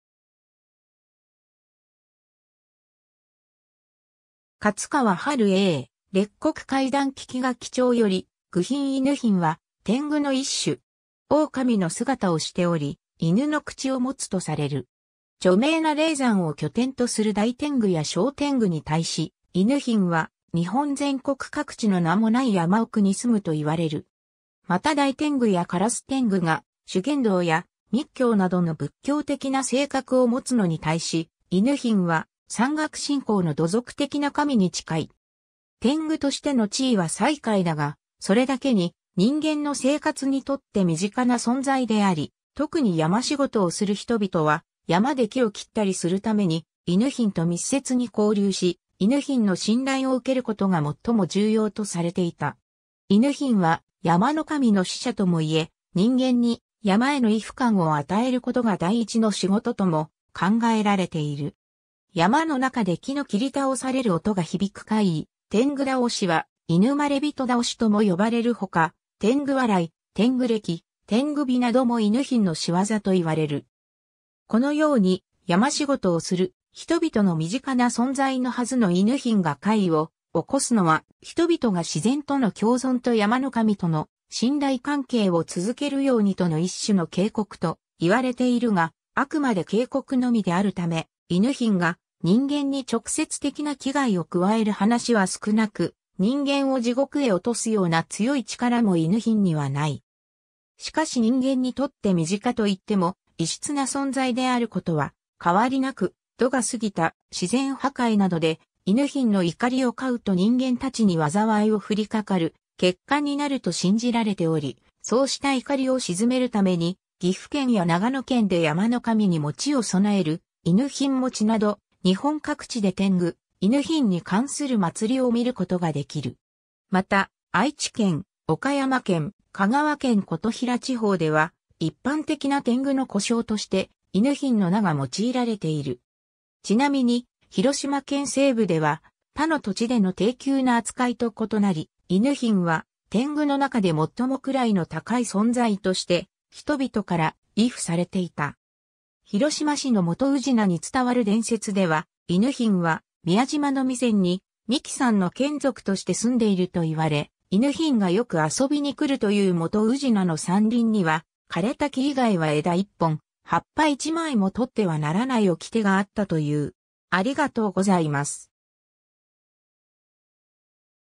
狗賓
勝川春英『列国怪談聞書帖』より「ぐひん」 狗賓（ぐひん）は、天狗の一種。
狼の姿をしており、犬の口を持つとされる。
 著名な霊山を拠点とする大天狗や小天狗に対し、狗賓は日本全国各地の名もない山奥に棲むといわれる。
また大天狗や烏天狗が修験道や密教などの仏教的な性格を持つのに対し、狗賓は山岳信仰の土俗的な神に近い。
天狗としての地位は最下位だが、それだけに人間の生活にとって身近な存在であり、特に山仕事をする人々は、山で木を切ったりするために狗賓と密接に交流し、狗賓の信頼を受けることが最も重要とされていた。
 狗賓は山の神の使者ともいえ、人間に山への畏怖感を与えることが第一の仕事とも考えられている。
山の中で木の切り倒される音が響く怪異・天狗倒しは狗賓倒しとも呼ばれるほか、天狗笑い、天狗礫、天狗火なども狗賓の仕業といわれる。
このように、山仕事をする人々の身近な存在のはずの狗賓が怪異を起こすのは、人々が自然との共存と山の神との信頼関係を続けるようにとの一種の警告といわれているが、あくまで警告のみであるため、狗賓が人間に直接的な危害を加える話は少なく、人間を地獄へ落とすような強い力も狗賓にはない。
 しかし人間にとって身近といっても、異質な存在であることは変わりなく、度が過ぎた自然破壊などで狗賓の怒りを買うと人間たちに災いを振りかかる結果になると信じられており、そうした怒りを鎮めるために岐阜県や長野県で山の神に餅を供える狗賓餅など、日本各地で天狗・狗賓に関する祭りを見ることができる。
 また、愛知県、岡山県、香川県琴平地方では、一般的な天狗の呼称として狗賓の名が用いられている。
 ちなみに広島県西部では、他の土地での低級な扱いと異なり、狗賓は天狗の中で最も位の高い存在として人々から畏怖されていた。
広島市の元宇品に伝わる伝説では、狗賓は宮島の弥山に三鬼さんの眷属として住んでいると言われ、狗賓がよく遊びに来るという元宇品の山林には、枯れた木以外は枝一本、葉っぱ一枚も取ってはならない掟があったという。